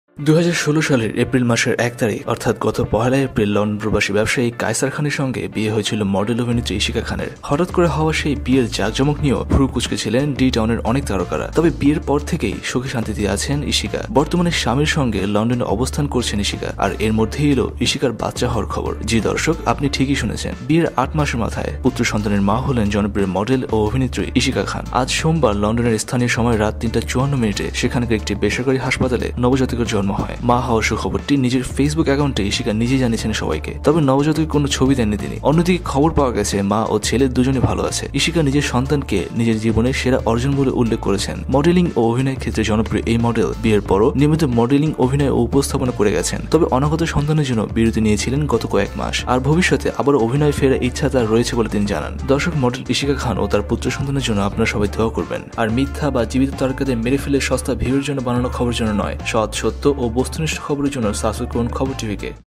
বিয়ের ৮ মাসেই বাচ্চার মা হলেন মডেল ইশিকা খান! । Model Ishika Khan becomes a Mother!
২০১৬ সালের এপ্রিল মাসের ১ তারিখ অর্থাৎ গত ১ এপ্রিল লন্ডনপ্রবাসী ব্যবসায়ী কায়সার খানের সঙ্গে বিয়ে হয়েছিল মডেল অভিনেত্রী ইশিকা খানের। হটাত করে হওয়া সেই বিয়ের জাঁকজমক নিয়েও ভুরু কুঁচকেছিলেন অনেক তারকা। তবে বিয়ের পর থেকেই সুখে শান্তিতেই রয়েছেন ইশিকা। বর্তমানে...
বিস্তারিতঃ